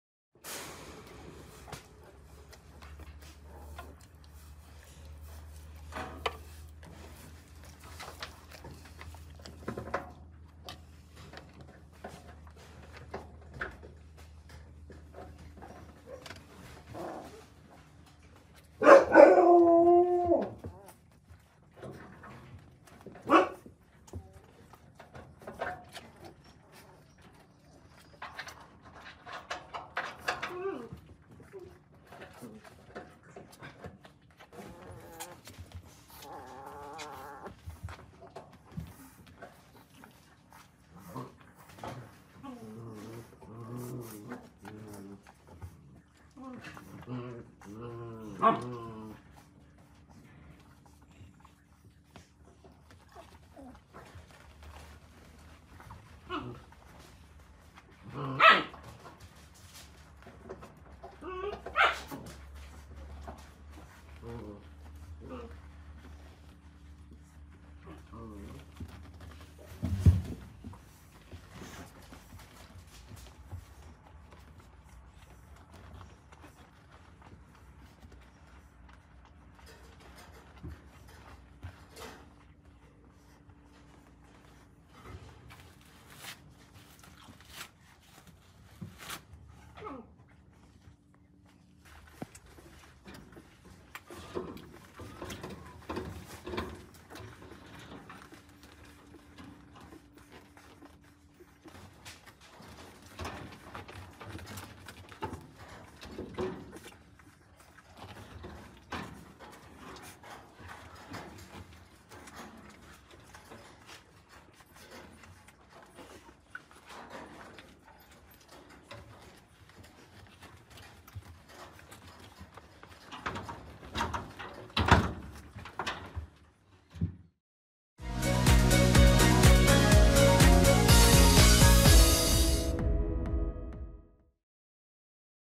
秋田犬の家族＃12 仲良く並んでおっぱいタイム
秋田犬犬舎 能代幸寿荘（のしろこうじゅそう）で、
11月に生まれた秋田犬の赤ちゃんの成長記録です。
ママがお散歩から戻ってきました。
お昼寝中だった赤ちゃんたちは、いそいそとママのそばに駆け寄ります。
きょうだいも合流して、仲良く並んでおっぱいタイムが始まりました。
一生懸命飲む姿が愛おしく、そして癒されます。
心温まる命の物語、次回もお楽しみに。